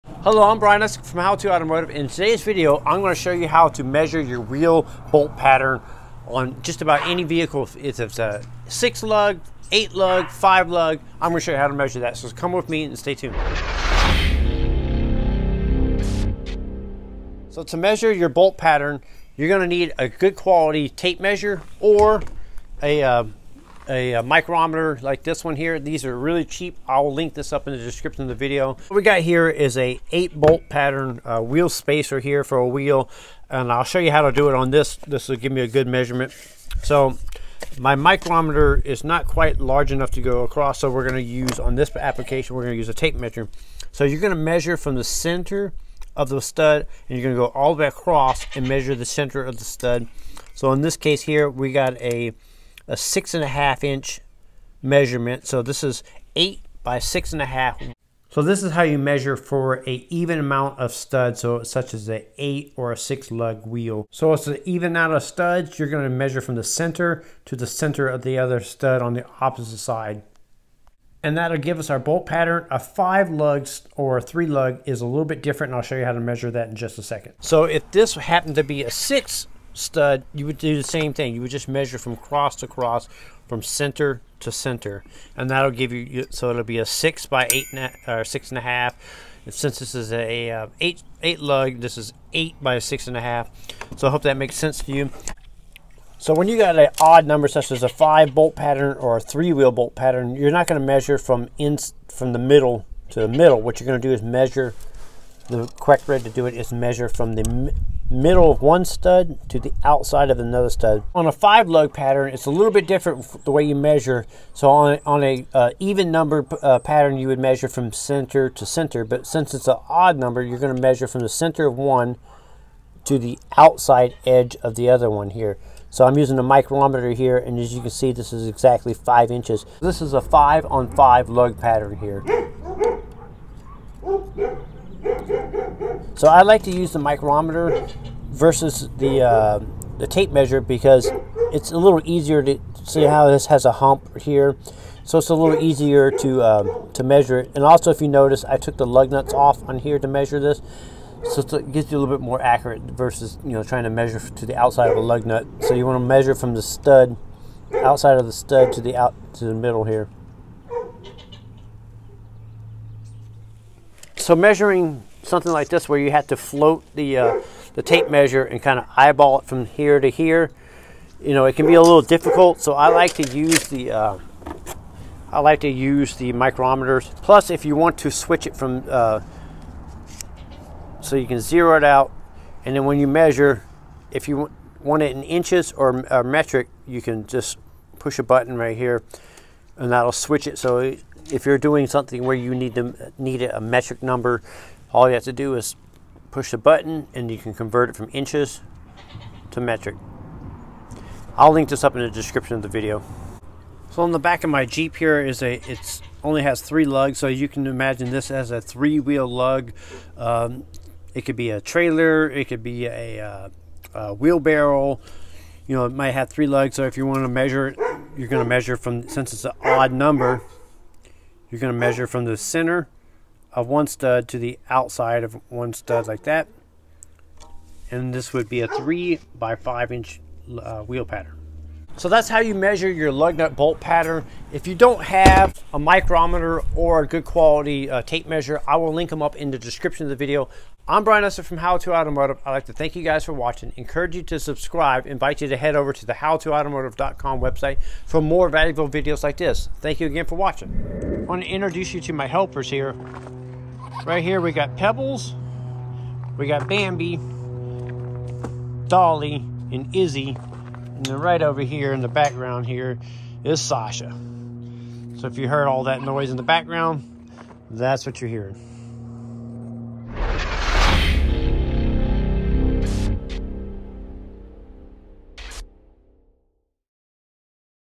How To Measure Any Wheel's Lug Nut Bolt Pattern
Brian Eslick from How to Automotive walks you step-by-step through the process of measuring the lug nut bolt pattern on any wheel.